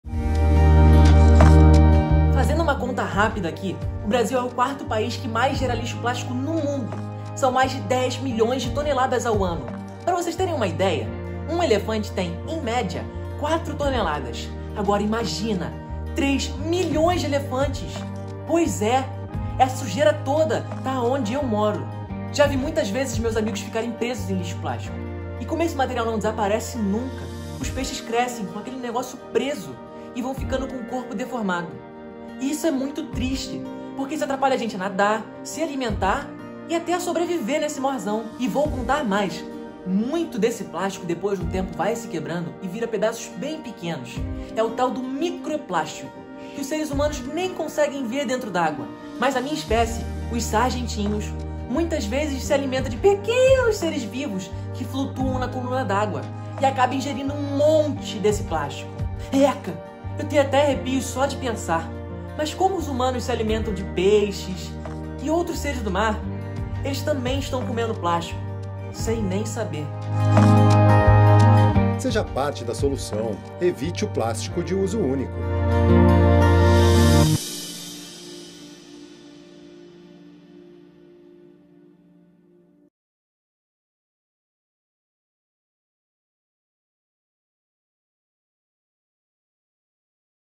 VIDEO PEIXE CAMPANHA POUIÇÃO MARINHA
Esse vídeo faz parte da campanha de sensibilização sobre poluição marinha nas Ilhas Cagarras e Águas do Entorno, um Ponto de Esperança reconhecido pela aliança mundial Mission Blue e promovido pelo Projeto Ilhas do Rio e Projeto Verde Mar